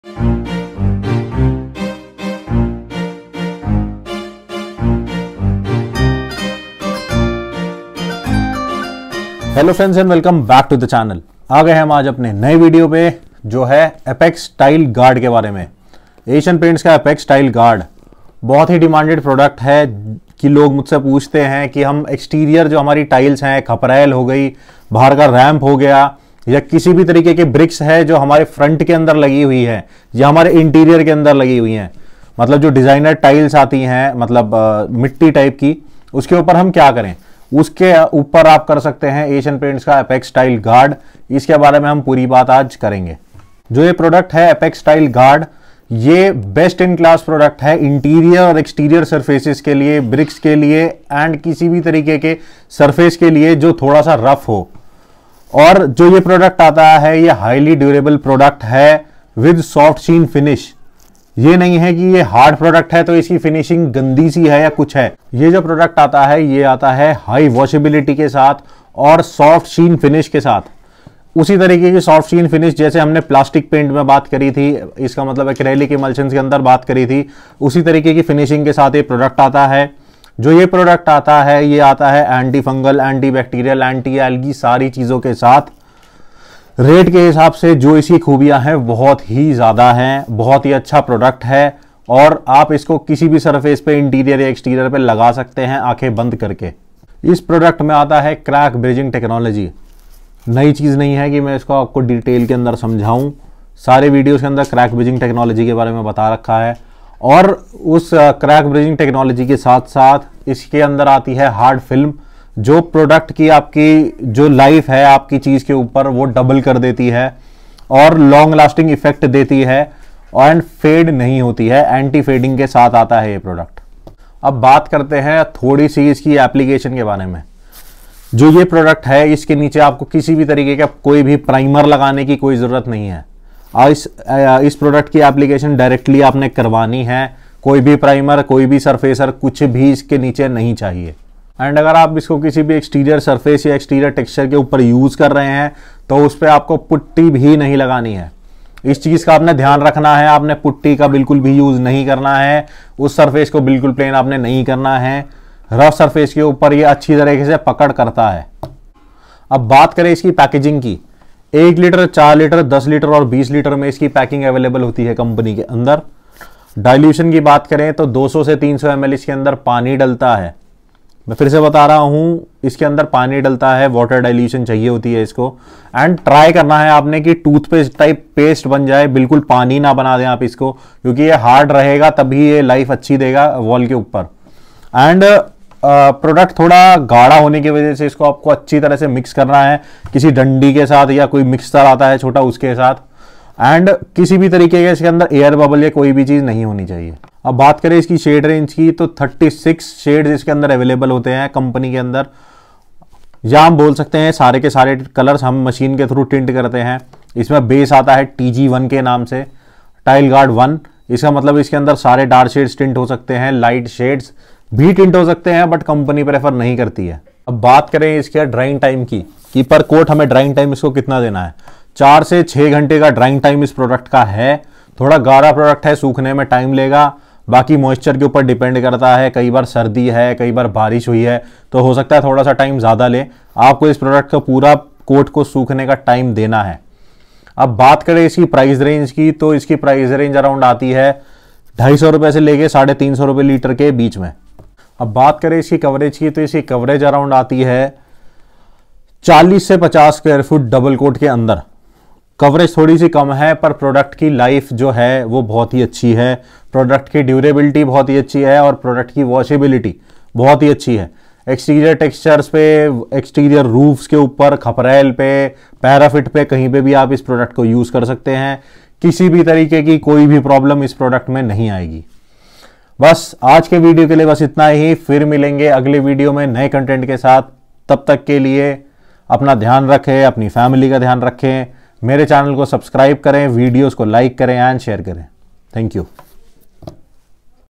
ASIAN PAINTS APEX TILE GUARD/TILE GUARD/PAINT FOR RAMPS,PAINT FOR BRICKS
This video is about Asian paints apex Tile Guard One of the best in class products for interior and exterior surfaces,Ramps,Brick tiles etc.Product with many benifits .